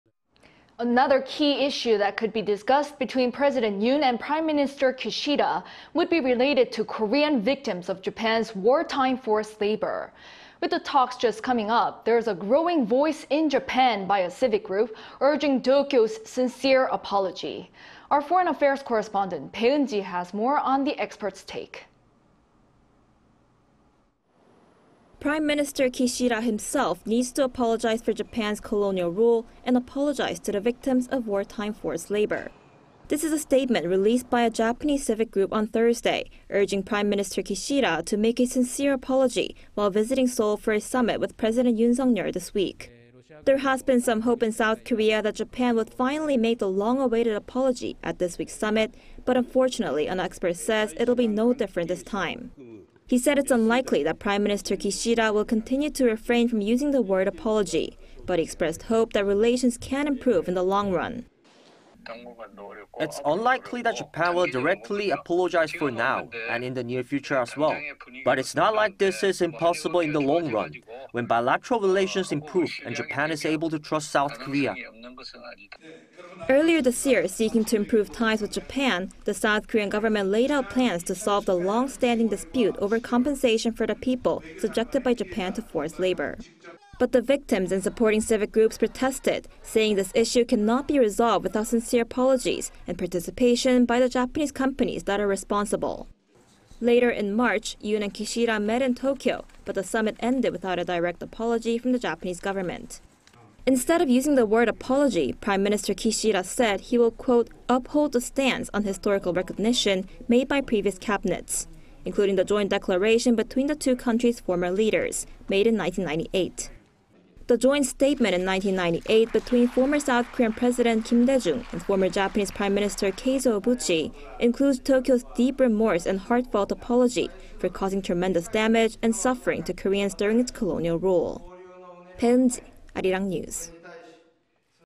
Will Japan show sincere apology for wartime forced labor and colonizing Korea?
尹-기시다, 7일 정상회담 이번 한일 정상회담서 얼마나 사과 메시지 담길까?

Another key issue that could be discussed between President Yoon and Prime Minister Kishida would be related to Korean victims of Japan's wartime forced labor.
With the talks just coming up there's a growing voice in Japan by a civic group urging Tokyo's sincere apology.
Our foreign affairs correspondent Bae Eun-ji has more on the experts' take.

'Prime Minister Kishida himself needs to apologize for Japan's colonial rule and apologize to the victims of wartime forced labor."
This is a statement released by a Japanese civic group on Thursday, urging Prime Minister Kishida to make a sincere apology while visiting Seoul for a summit with President Yoon Suk Yeol this week.
There has been some hope in South Korea that Japan would finally make the long-awaited apology at this week's summit, but unfortunately, an expert says it will be no different this time.
He said it's likely that Prime Minister Kishida will continue to refrain from using the word "apology," but he expressed hope that relations can improve in the long run.

"It's unlikely that Japan will directly apologize for now, and in the near future as well. But it's not like this is impossible in the long run --when bilateral relations improve, and Japan is able to trust South Korea."

Earlier this year, seeking to improve ties with Japan, the South Korean government laid out plans to solve the long-standing dispute over compensation for the people subjected by Japan to forced labor.
But the victims and supporting civic groups protested, saying this issue cannot be resolved without sincere apologies and participation by the Japanese companies that are responsible.
Later, in March, Yoon and Kishida met in Tokyo, but the summit ended without a direct apology from the Japanese government.
Instead of using the word "apology," Prime Minister Kishida said he will  "uphold" the stance on historical recognition made by previous cabinets, including the joint declaration between the two countries' former leaders, made in 1998.
The joint statement in 1998 between former South Korean President Kim Dae-jung and former Japanese Prime Minister Keizo Obuchi --includes Tokyo's "deep remorse and heartfelt apology" for causing tremendous damage and suffering to Koreans during its colonial rule.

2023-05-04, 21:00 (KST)